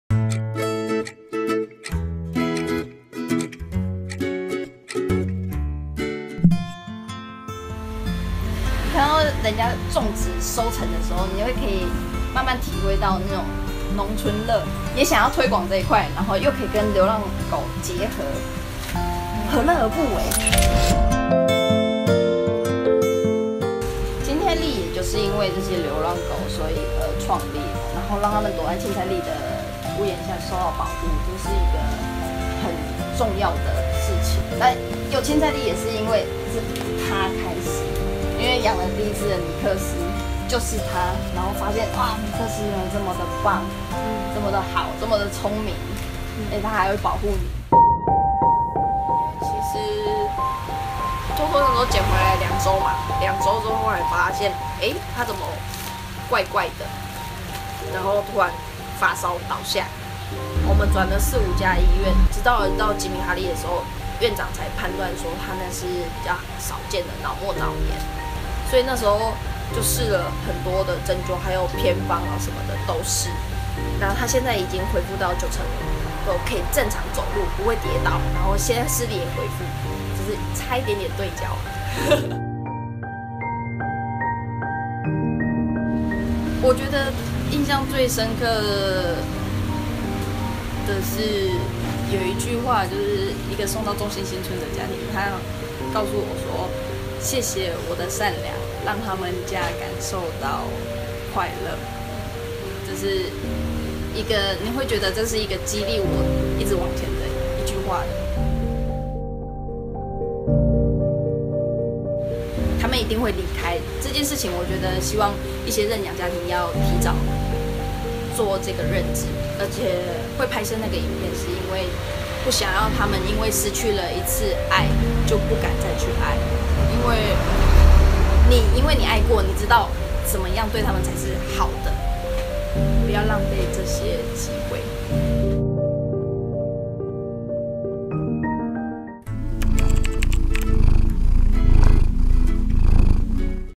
日日好日／青菜笠
本次日日好日採訪了「青菜笠」
這個28歲的女孩很不一樣，她種菜賣菜就是為了救流浪動物。